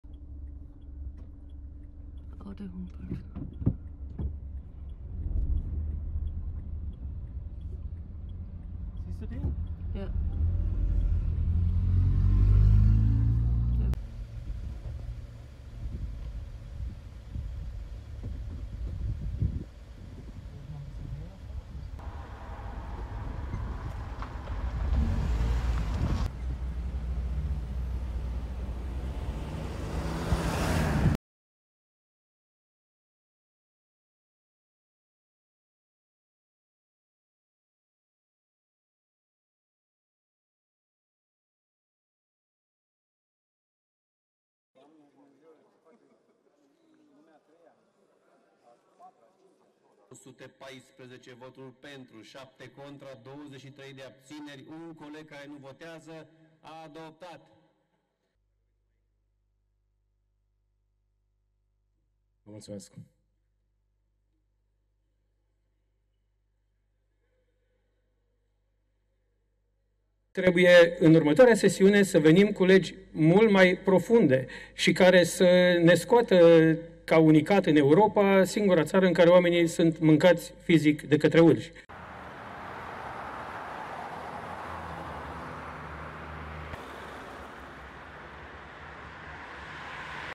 Romanian lawmakers more than double bear kill quotas to prevent attacks | ABS-CBN News
Romanian lawmakers more than doubled the annual number of legal bear kills on Monday (July 15) to control the population and avoid further attacks against people.

The emergency parliamentary session followed the death last week of a 19-year-old hiker who was attacked by a bear on a popular trail in the Carpathian mountains in central Romania. The country has Europe's largest population of brown bears outside Russia.

The European Union state has annual quotas to proactively control the size of the bear population and to remove animals that have become accustomed to entering cities in search of food.

In 2023, the quotas amounted to 220 annual bear kills. On Monday, lawmakers increased the number to 481 per year.

With hundreds of bear sightings each year, authorities are struggling to keep residents and tourists in mountain towns safe from the wild animals. Local media regularly report bear attacks on people and livestock. — Report from Reuters